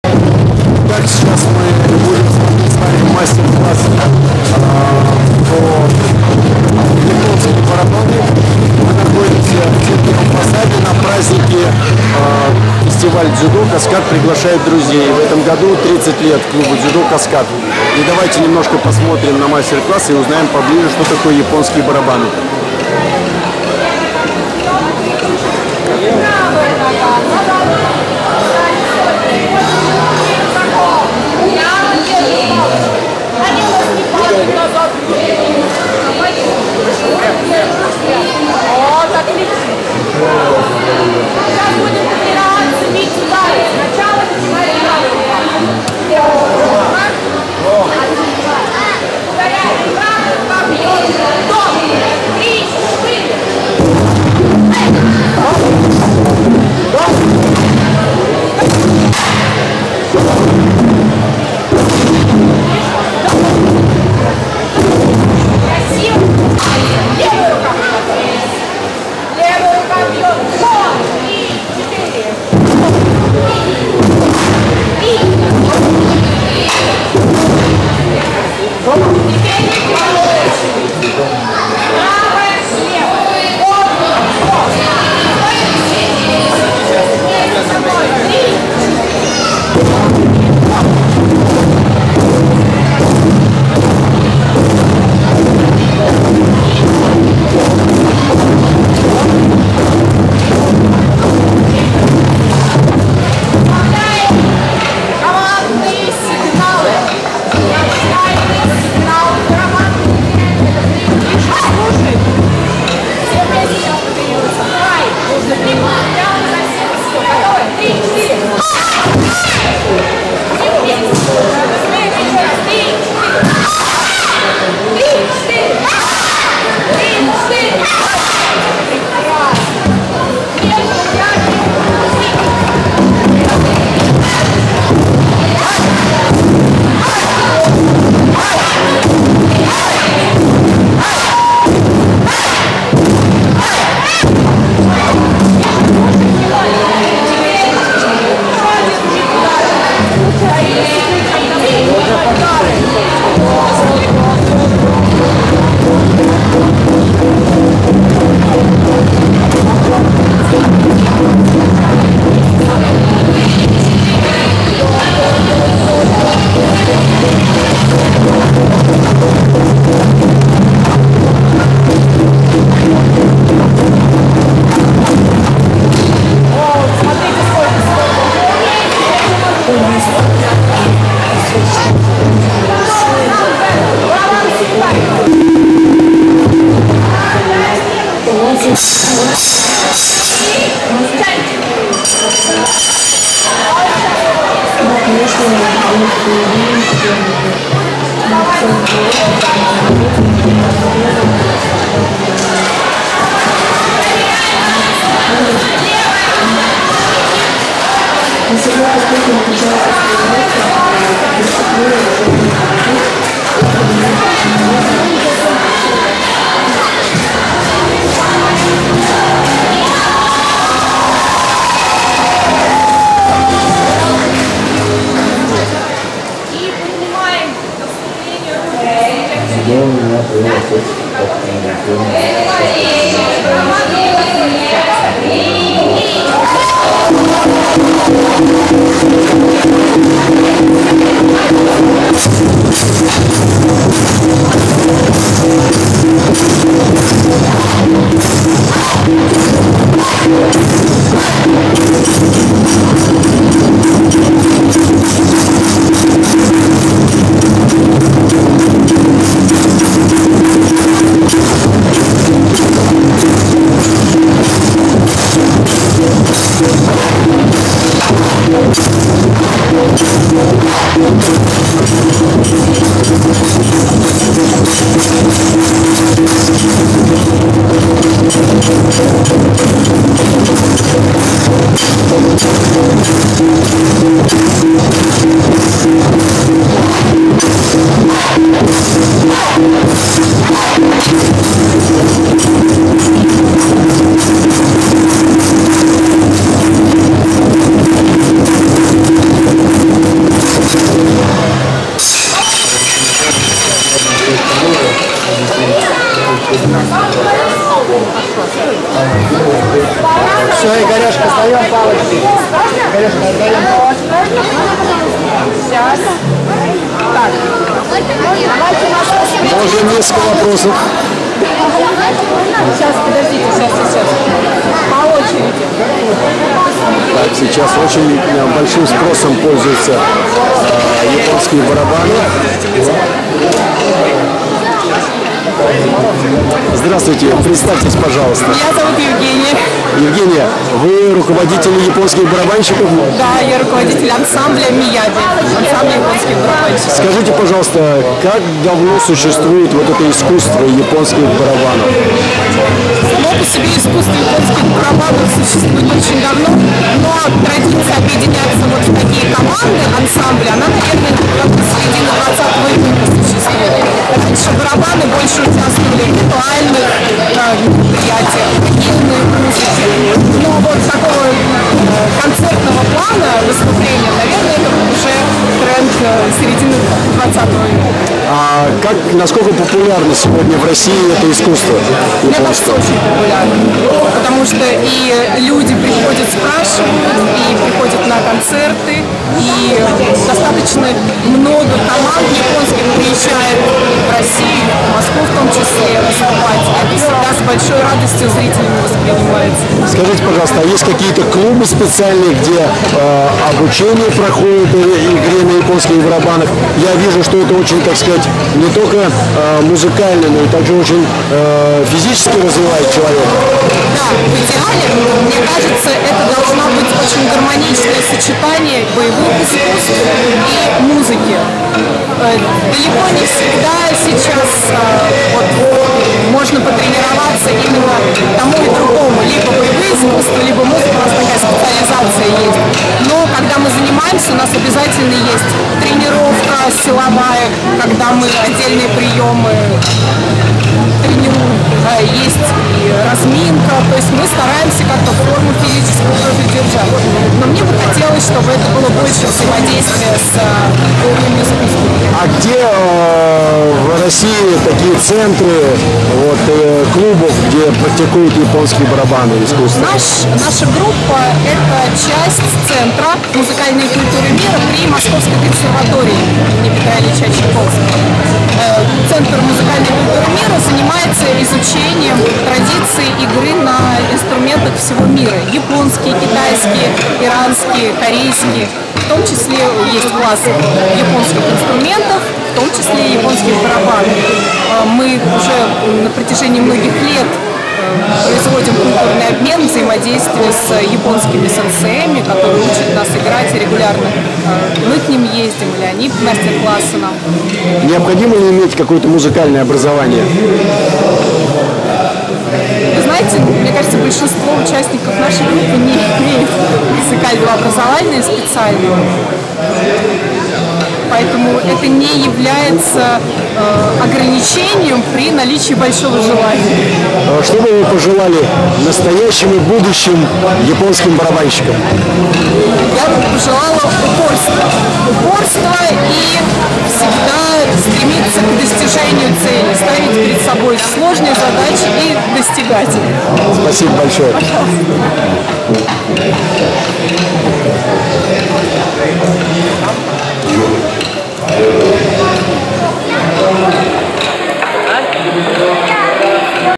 АНСАМБЛЬ ЯПОНСКИХ БАРАБАНОВ МИЯБИ/ ЕЛЕНА ГЛУХОВА/ ВЫСТУПЛЕНИЕ И ИНТЕРВЬЮ НА ФЕСТИВАЛЕ ДЗЮДО
В рамках Фестиваля дзюдо "Каскад приглашает друзей" состоялось выступление ансамбля японской музыки и японских барабанов "Мияби", руководитель Евгения Глухова.